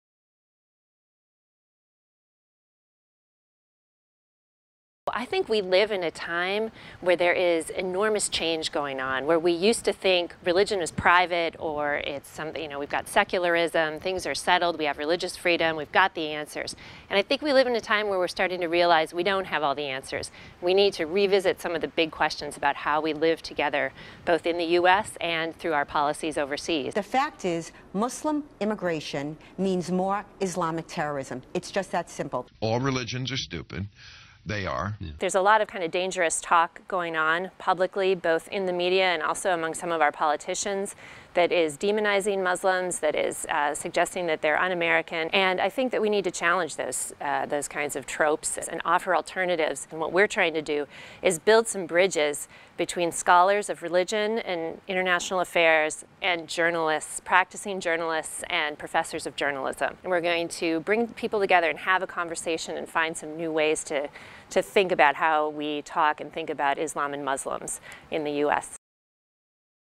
Faculty Spotlight: Elizabeth Shakman Hurd
Northwestern University political science professor and Buffett Institute faculty fellow Elizabeth Shakman Hurd talks about reshaping the image of religion in America. Learn more about her work at the Buffett Institute